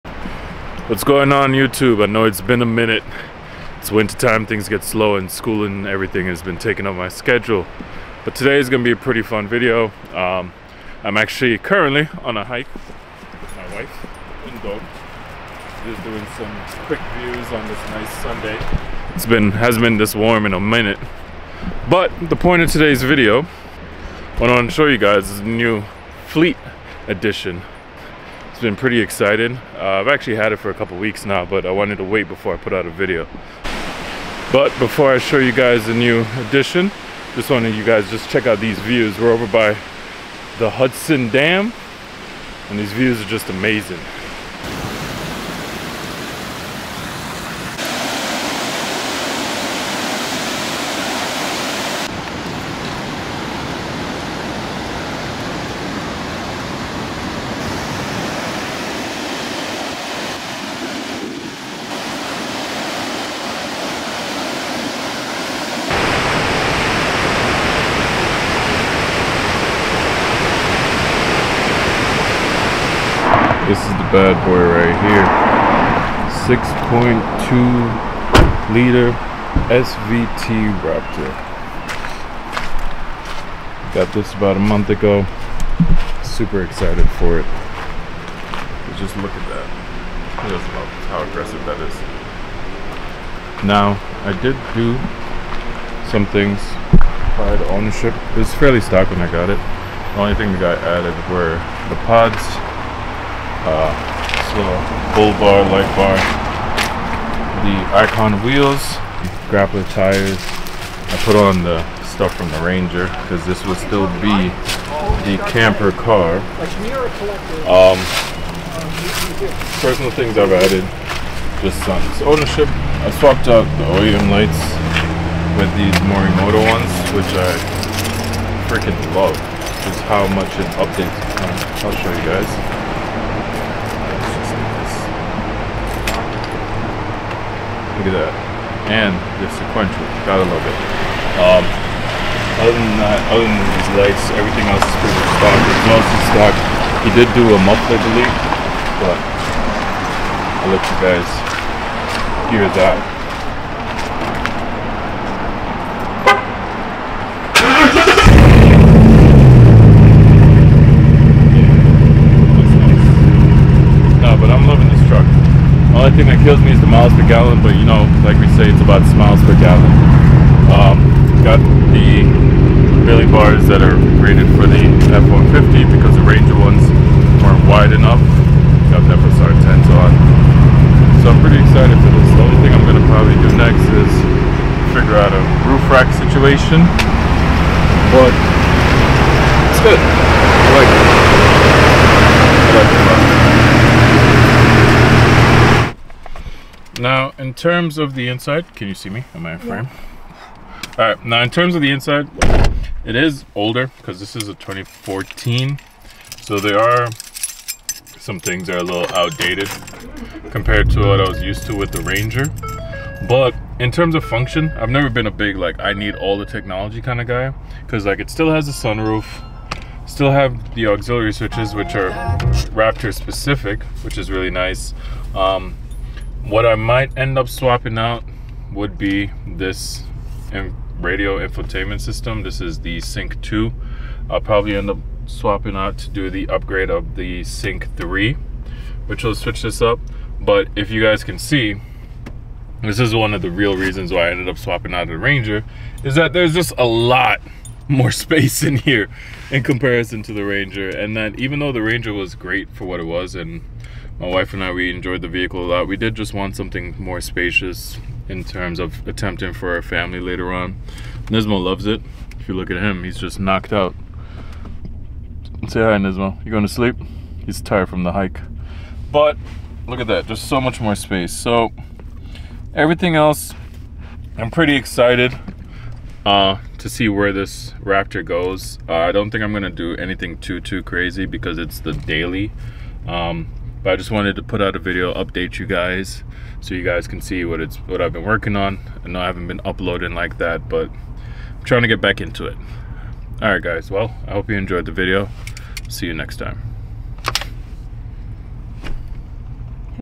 Selling My Ford Ranger After 2 Years
Hey guys, just a quick life update because it's been a while since I've uploaded.
Recently sold my Ford Ranger after 2 1/2 years of ownership.
I had zero issues with the truck but I always found myself longing for something bigger.
This video is just a quick intro of those changes